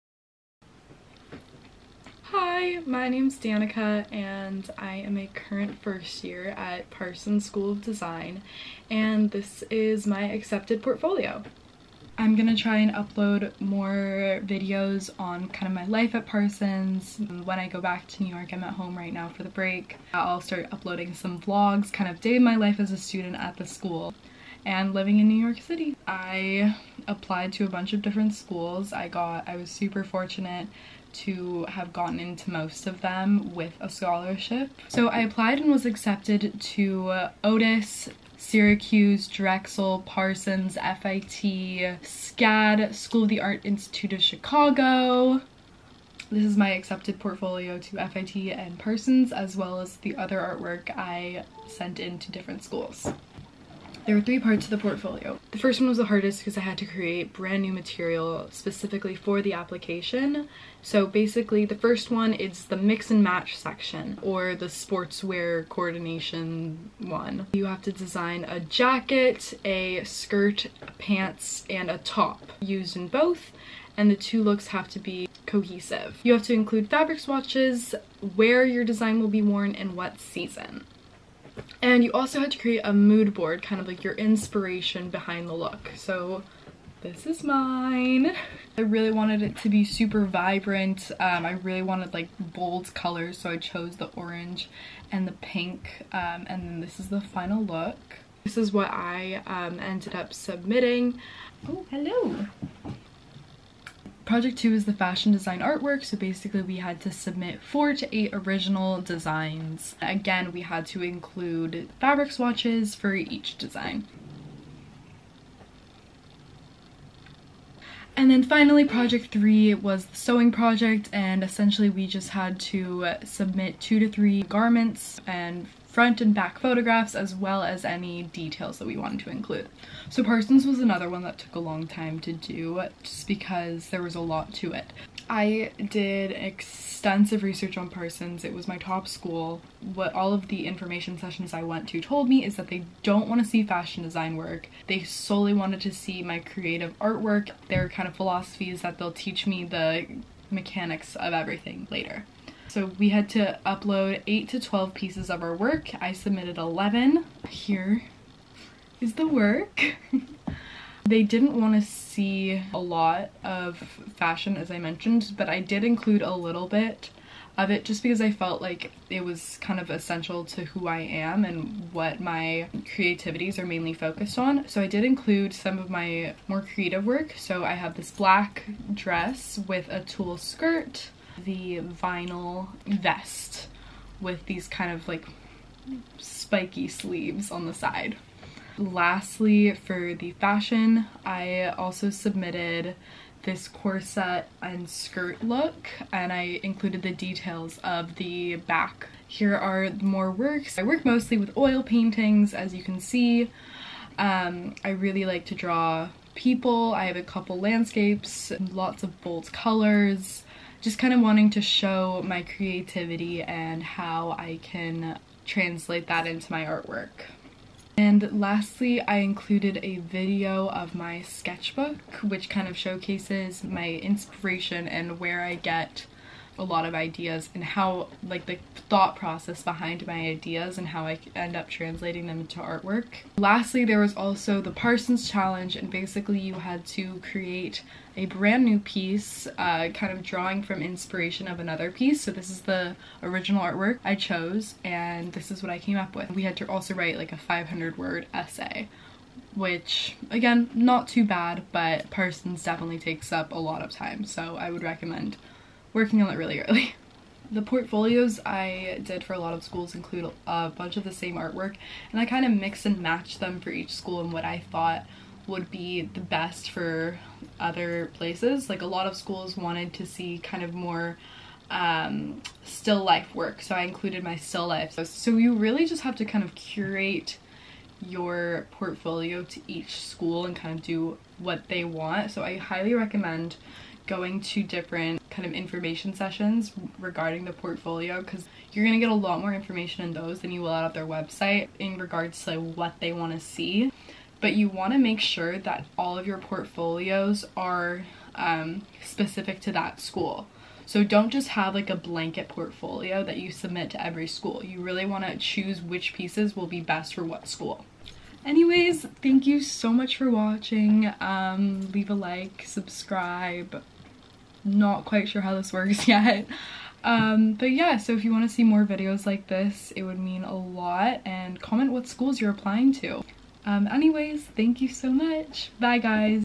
My accepted fashion design portfolio (Parsons, FIT, & more)
Hi!!! This is my accepted art and fashion design portfolio to FIT, Parsons, and others as well as advice as to how to curate the portfolio application!